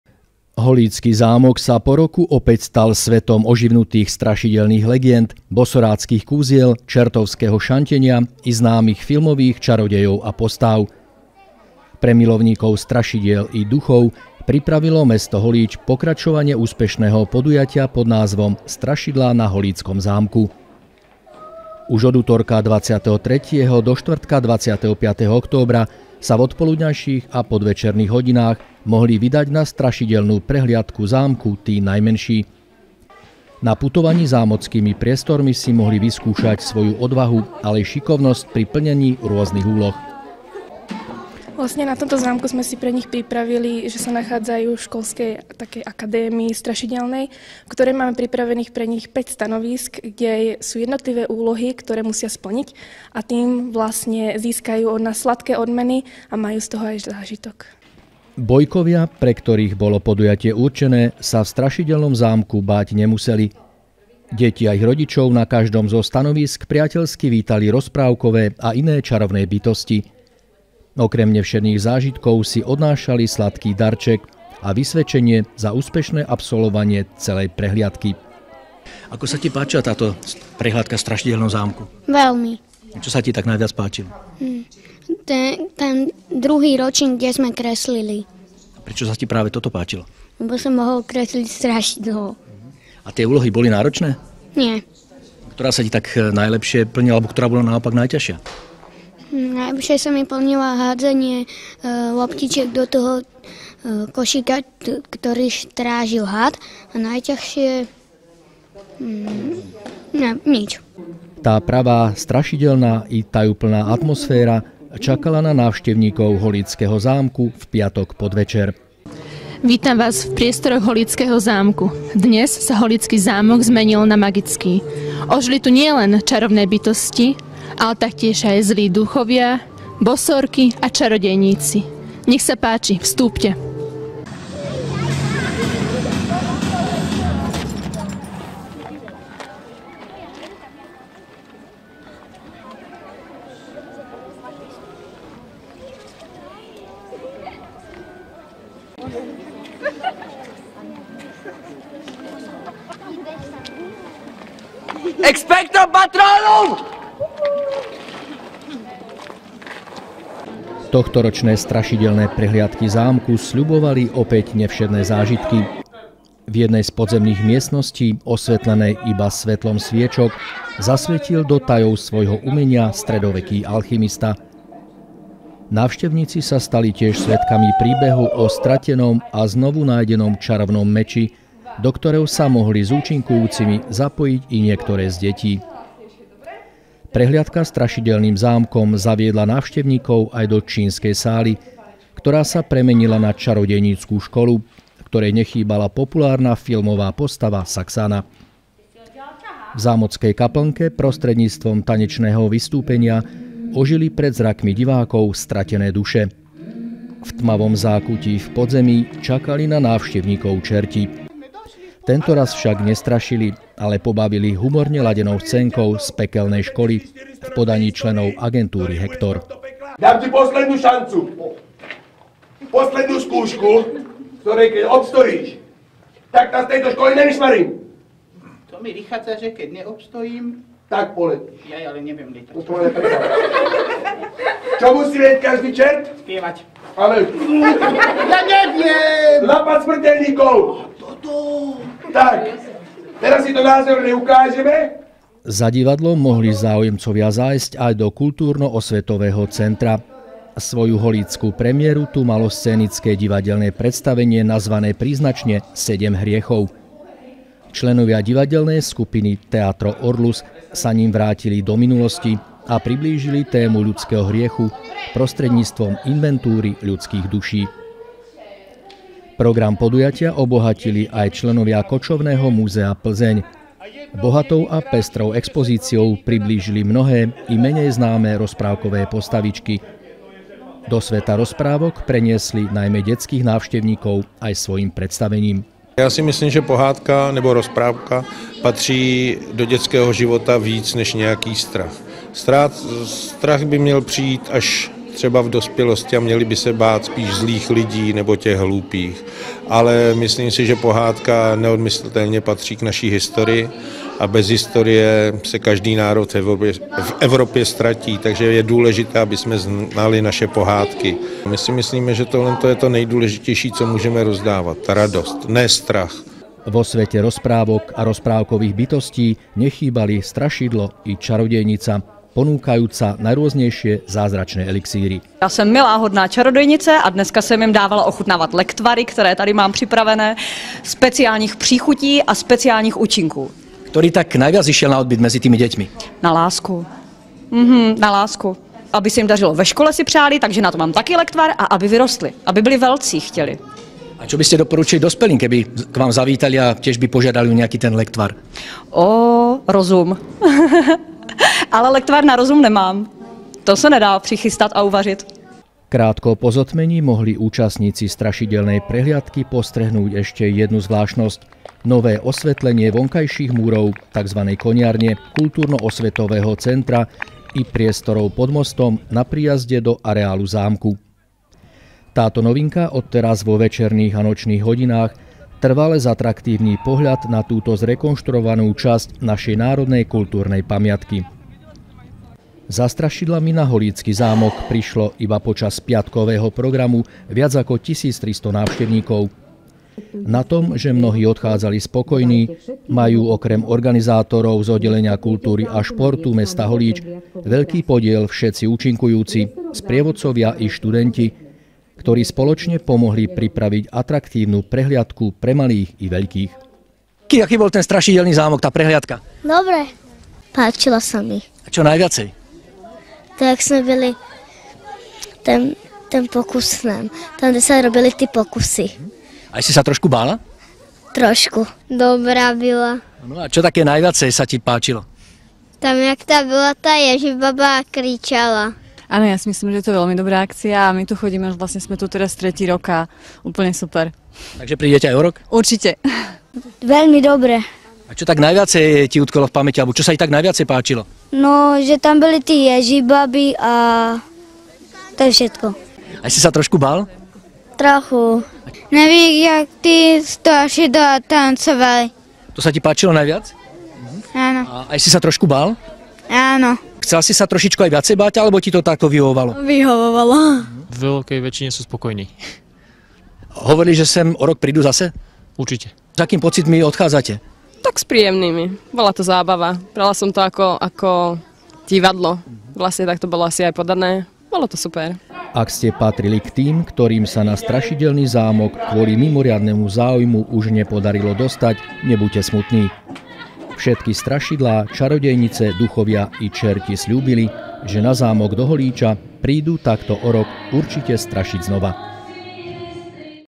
Na Holíčsky zámek se vrátili strašidla
Setkat se se strašidly? V Holíči na zámku to bylo možné.